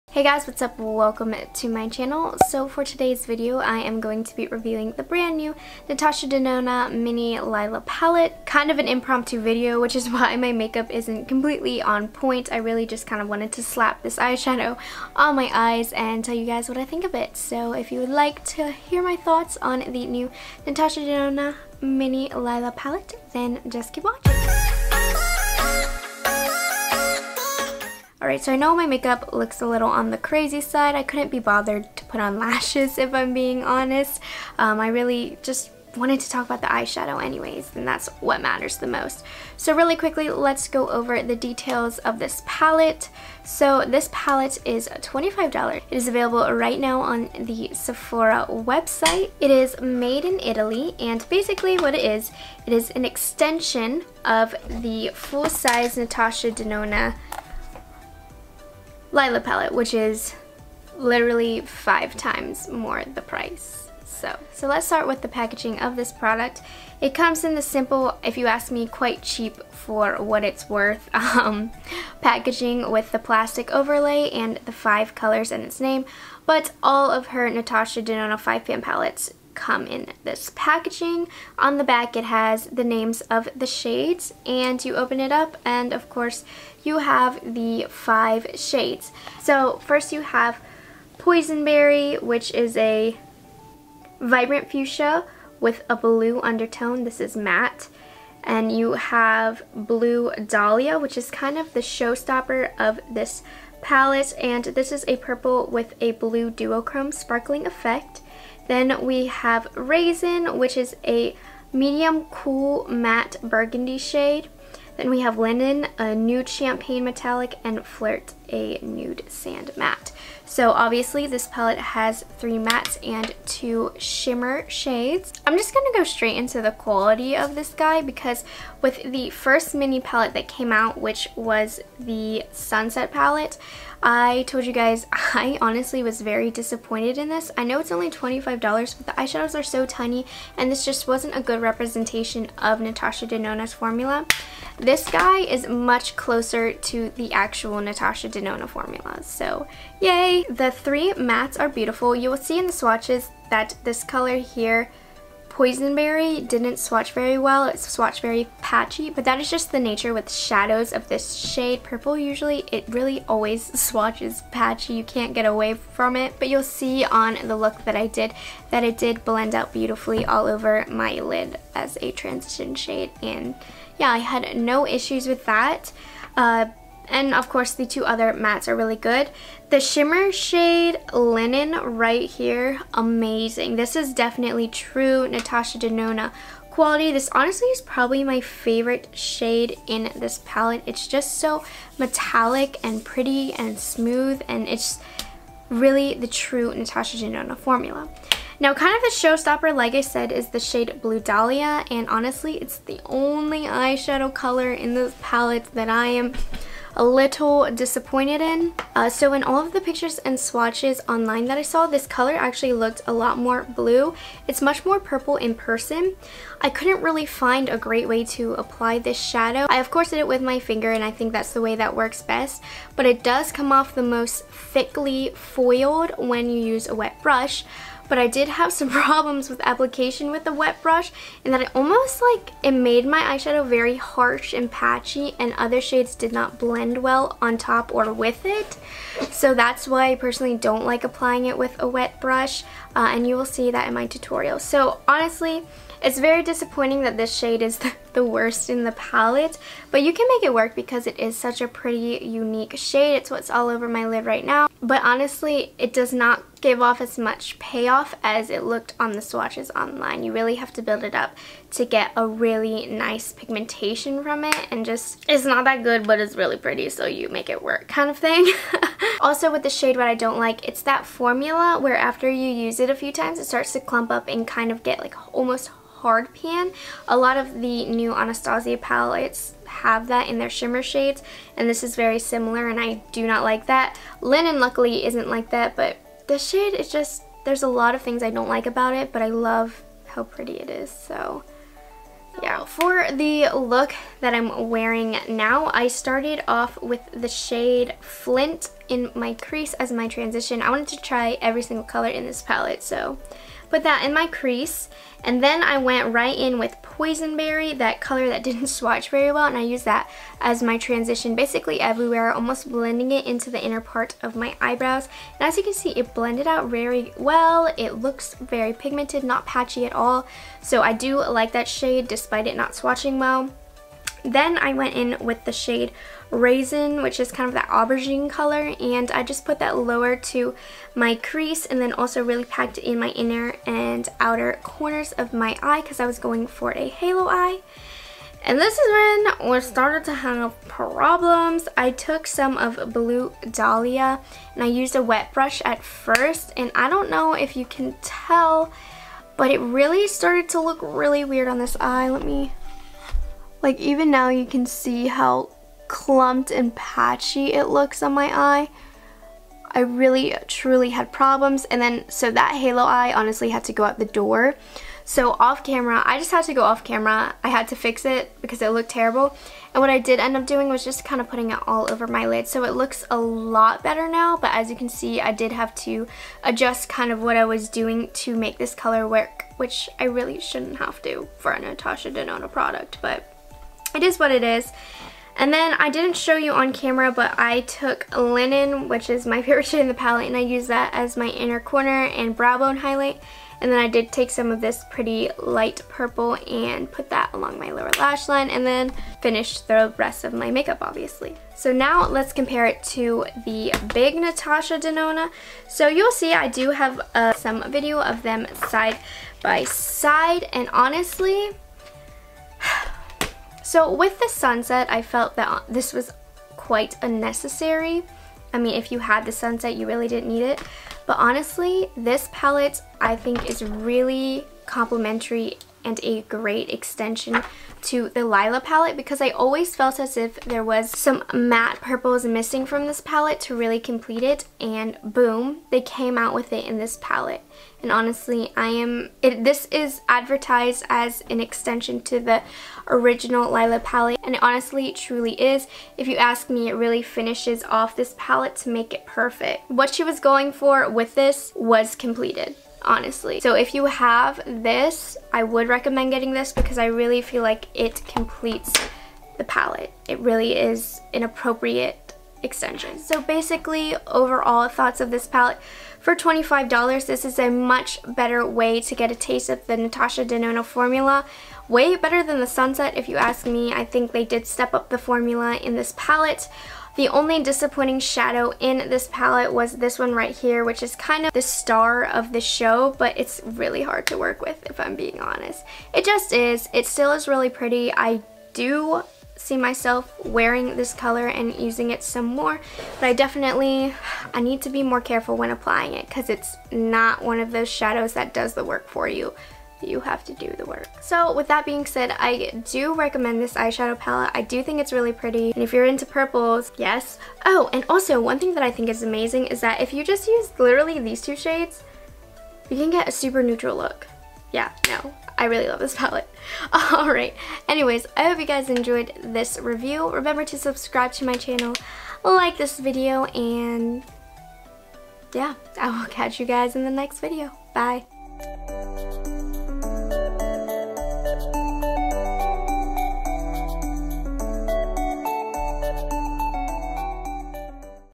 Natasha Denona MINI Lila Palette | Review, Swatches & Tutorial
In todays video I review the new Natasha Denona MINI Lila Palette!

What Im Wearing:
- YSL Touche Eclat All in One Glow in B30
- KKW Concealer in Shade 4
- Morphe Celebutante Brontour
- PUR Celebrate Your Selfie "Flirty" Blush
- ABH Medium Brown Brow Wiz
- Natasha Denona Mini Lila Palette
- NYX Dainty Daze Matte Suede Lipstick
- NYX Fortune Cookie Butter Gloss